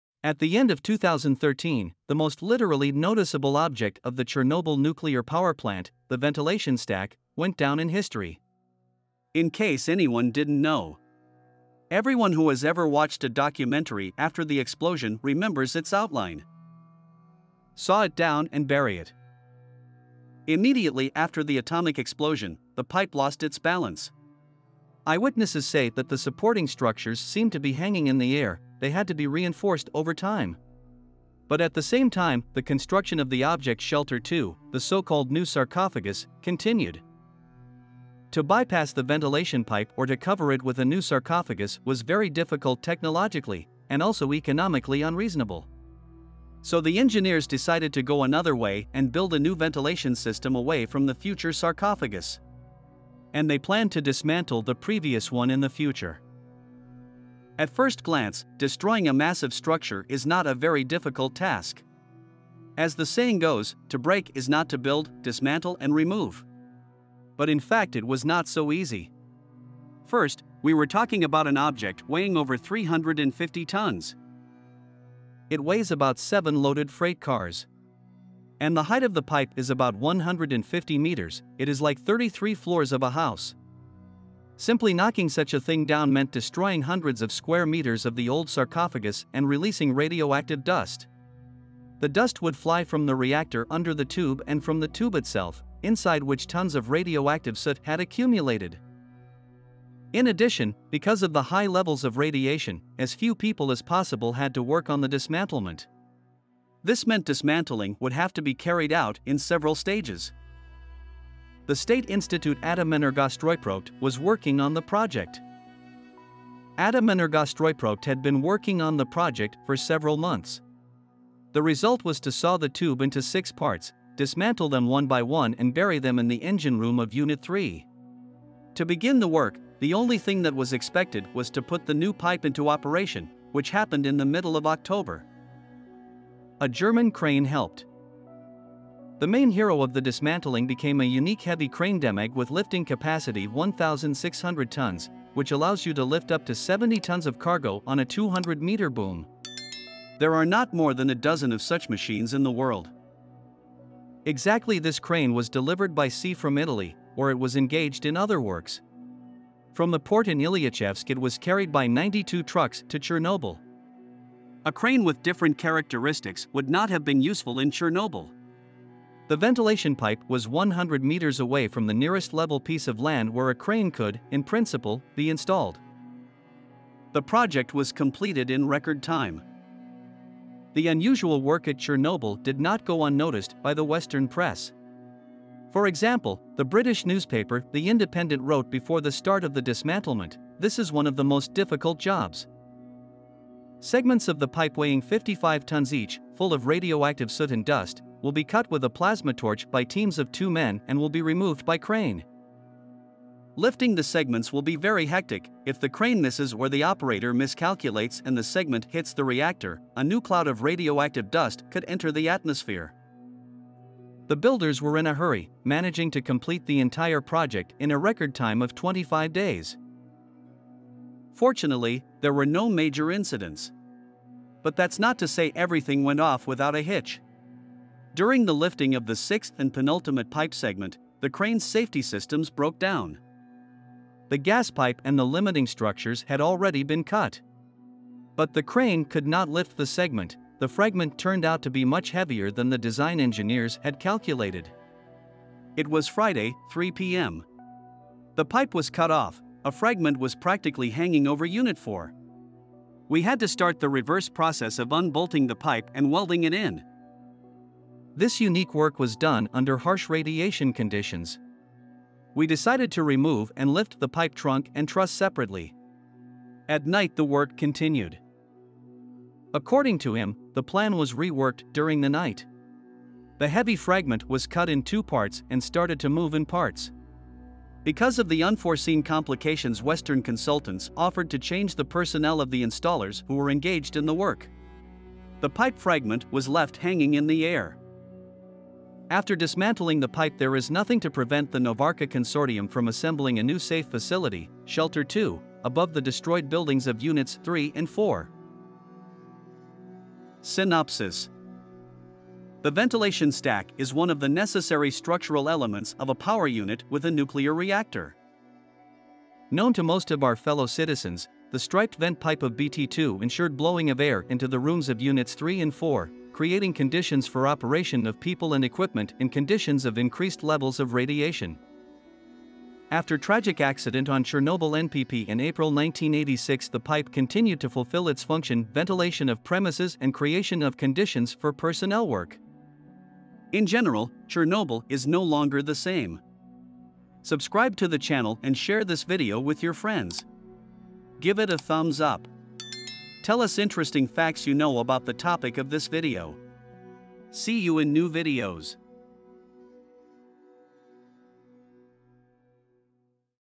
Chernobyl. Why was the pipe cut at the Chernobyl nuclear power plant
At the end of 2013, the most noticeable object of the Chernobyl nuclear power plant, the ventilation pipe, went down in history. Its outlines are remembered by everyone who has ever watched a documentary chronicle after the explosion.